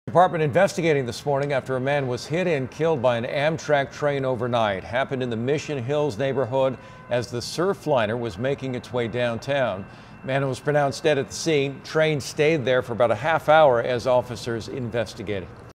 Man struck, killed by train in Mission Hills area
A man was pronounced dead at the scene after he was hit and killed by a train near Washington and Pacific Highway.